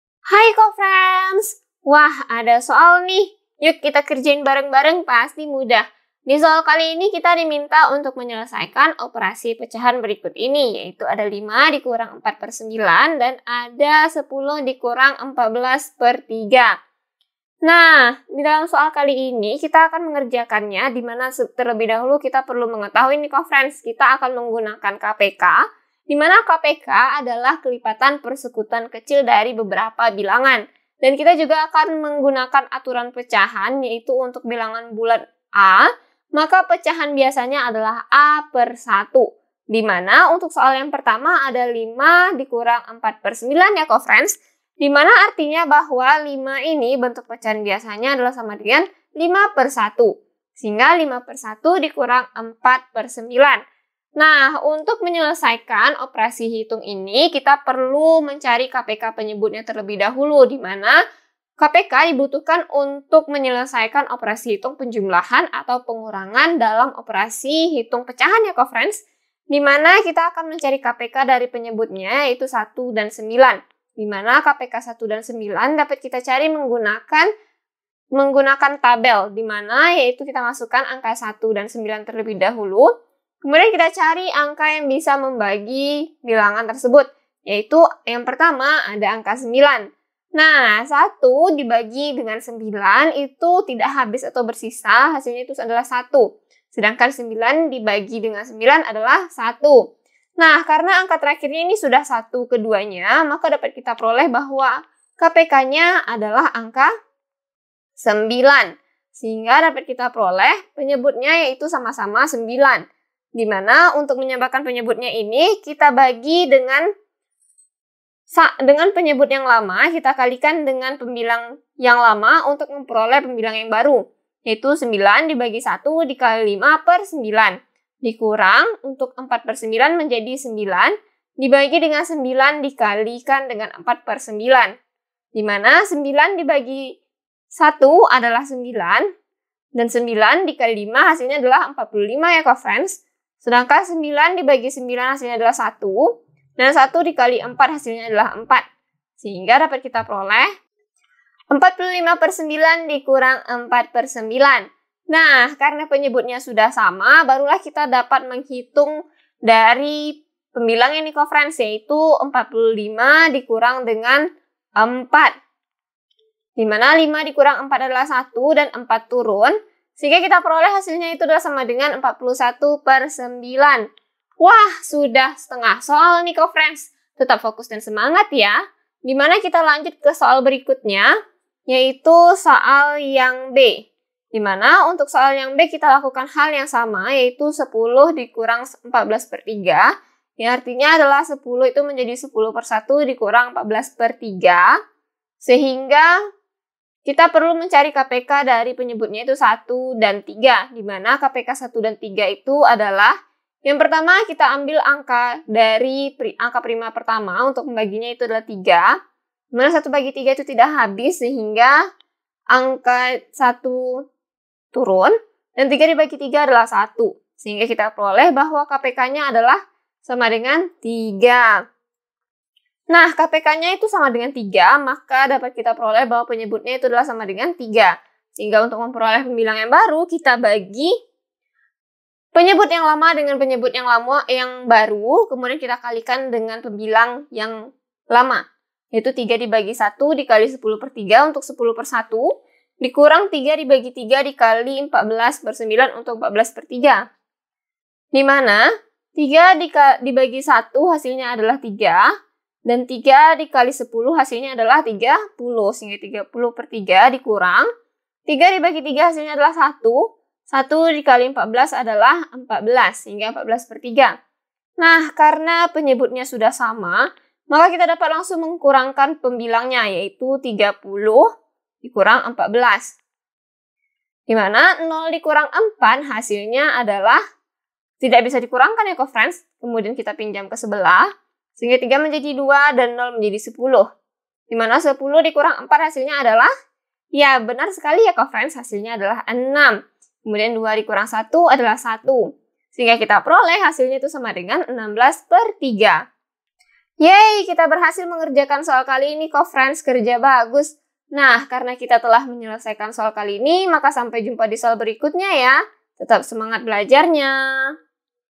9 5 - a. 14 b. 10 3
9 5 - a. 14 b. 10 3

Topik atau Materi: Penjumlahan dan Pengurangan Bilangan Pecahan Campuran - Operasi Bilangan Pecahan - Aritmatika - Matematika Kelas 5 SD - K-13 Revisi

Sumber Soal: Matematika Gasing Kelas 5, K-13 Revisi, YS, 2020 , PT Kandel

00:00 : Bedah Soal
01:00 : Konsep, rumus dan pengertian Penjumlahan dan Pengurangan Bilangan Pecahan Campuran
01:18 : Langkah penyelesaian soal
04:08 : Jawaban akhir
04:38 : Penutup